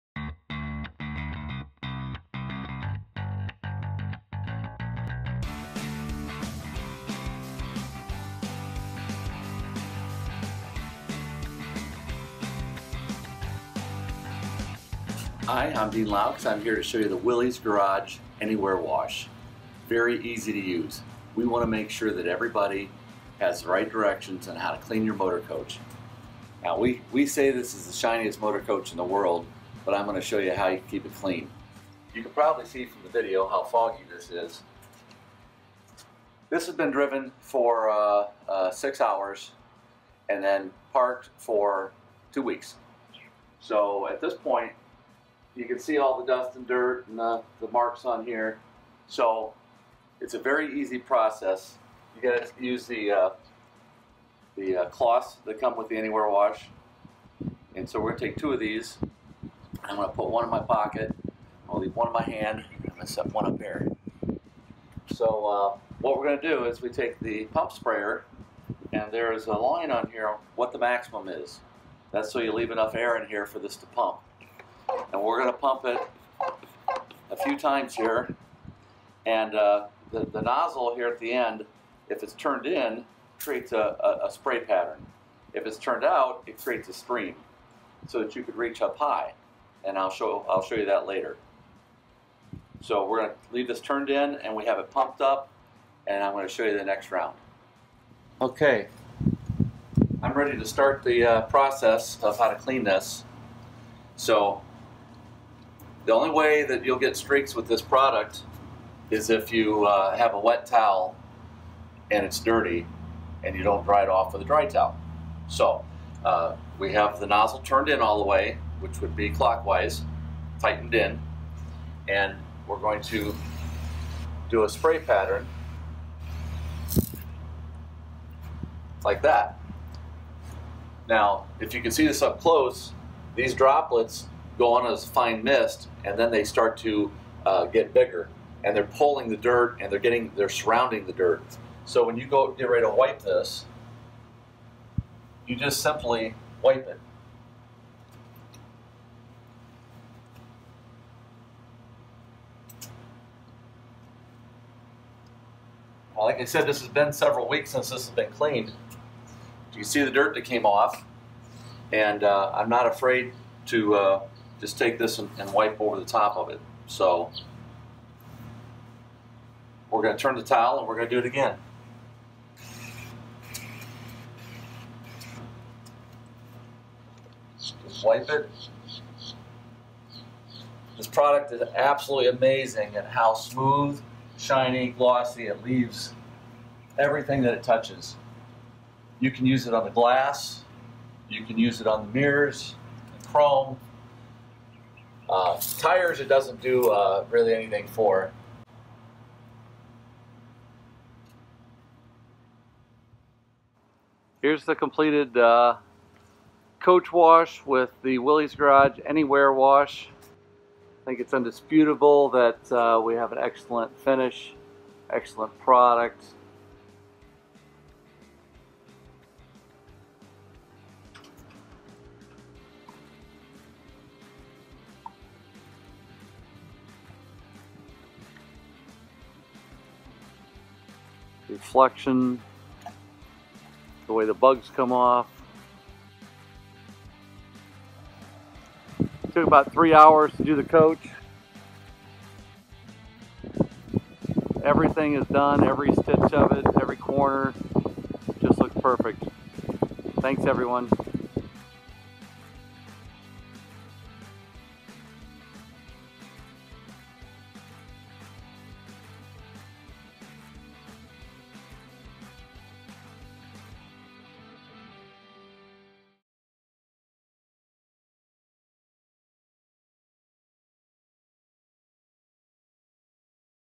How to Maintain the Shiniest Bus in the World
Dean Loucks from The Art of Design (TAOD) gives a quick tutorial on how he maintains the flawless look of his coach using Willy's Garage Anywhere Wash